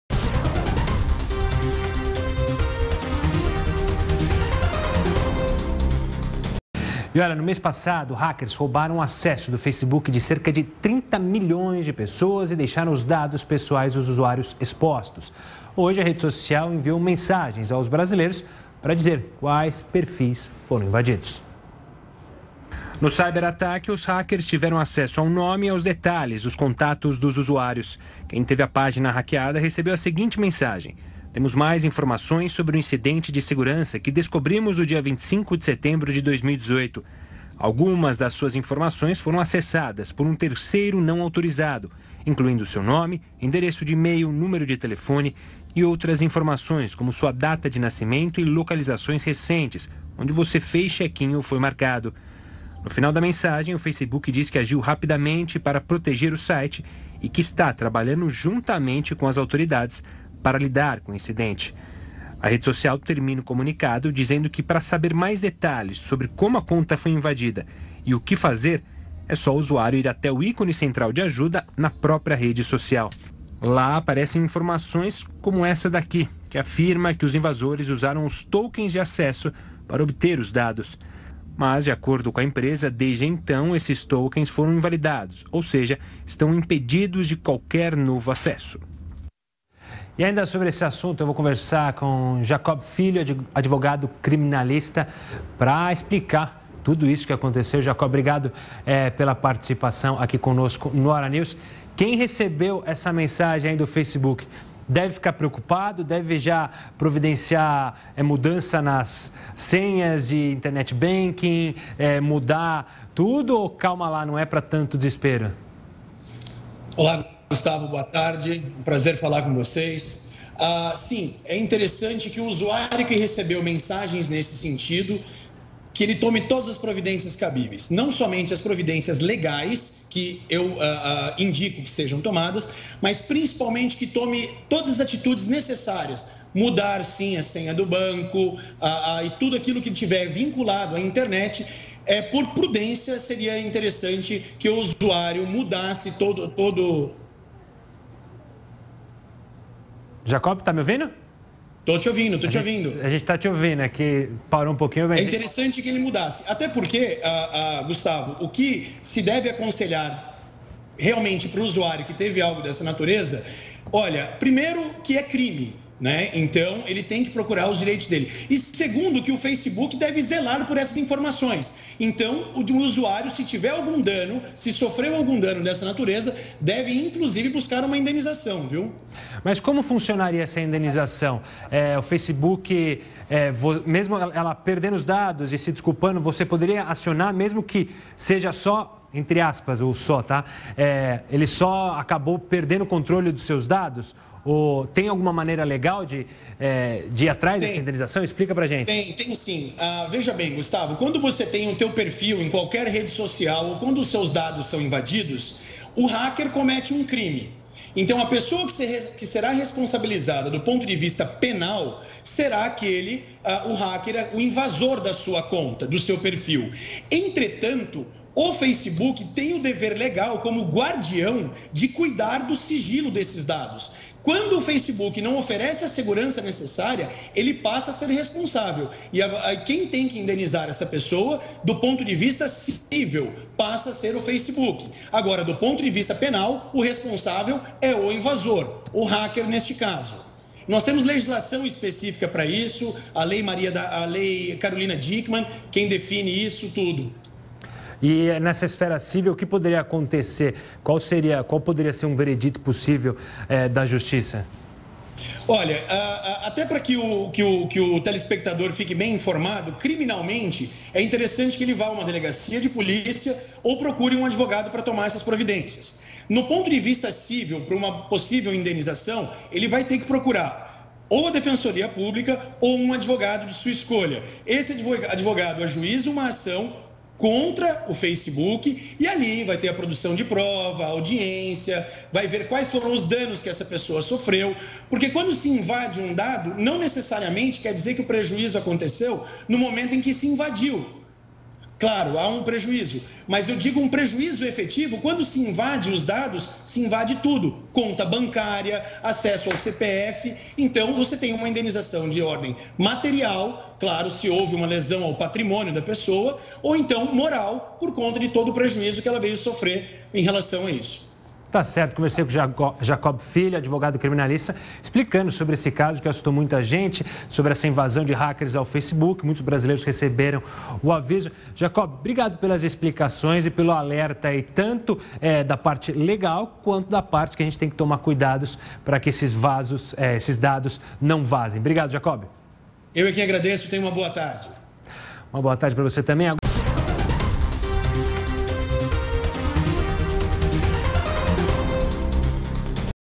O advogado Jacob Filho falou ao vivo na Record News sobre a invasão de contas dos usuários do Facebo
contato-advogado-criminalista